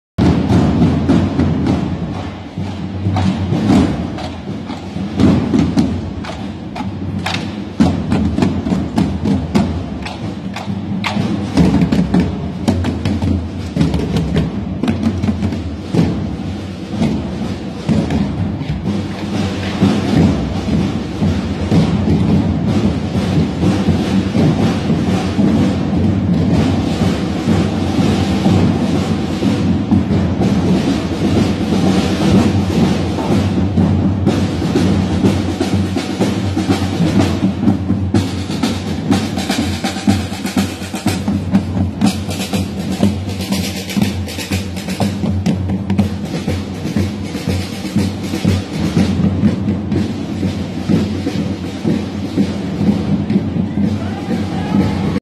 Medieval troops arrives in Pisa - Bollywood film shooting (Amour - Prabhas)
Recorded by Joeni Arianto Kurniawan